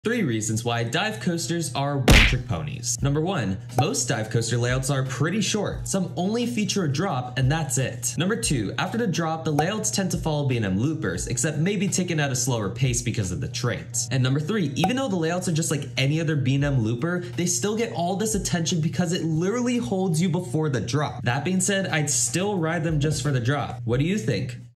3 Reasons Why Dive Coasters are 1-TRICK PONIES
Ever wondered why coaster enthusiasts aren’t as crazy over dive coasters as you would’ve expected? Well, here’s your answer!